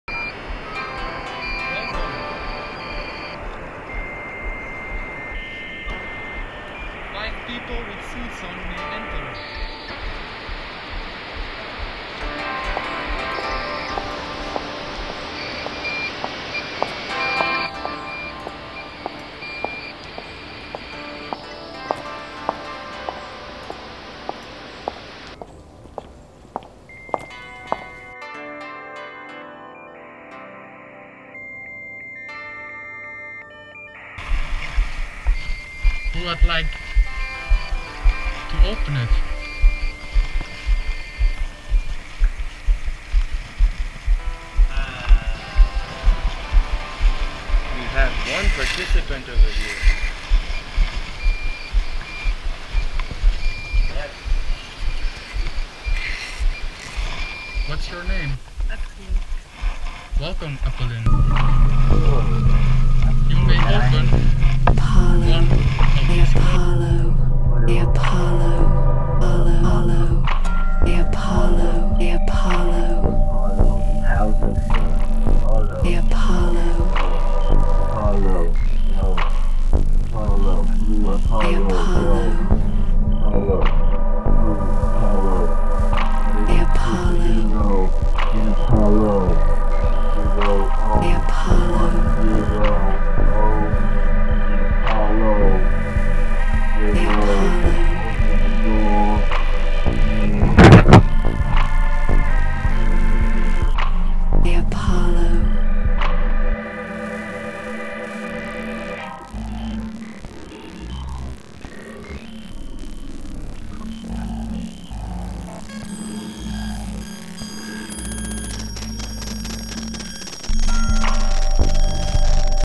Art-performance: DO NOT OPEN 01 // CHPT.01 - DO NOT OPEN GALLERY (2017)
This is an experimental aftermovie art-recording made by Vinco Zierowan mixed with imagery by Jasper Flikschuh. Thisi aftermovie shows a brief insight in the experimental art-happening organized in 2017 by the art-collective CHPT.01